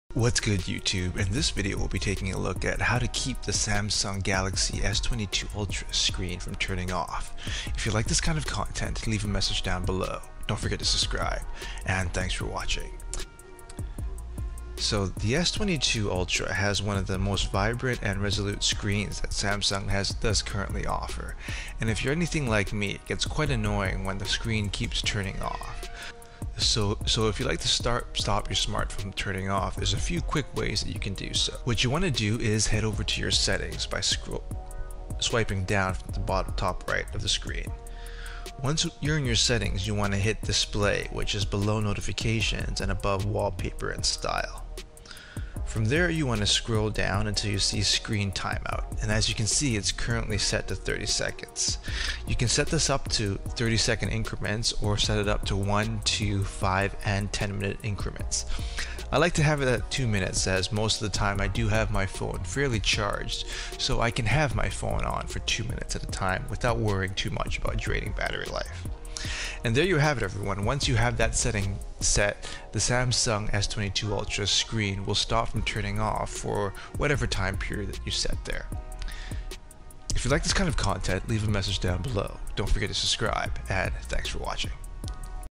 Samsung S22 Ultra - How to stop the screen from turning off.
This video will go over how to stop the screen on the Samsung S22 Ultra from turning off.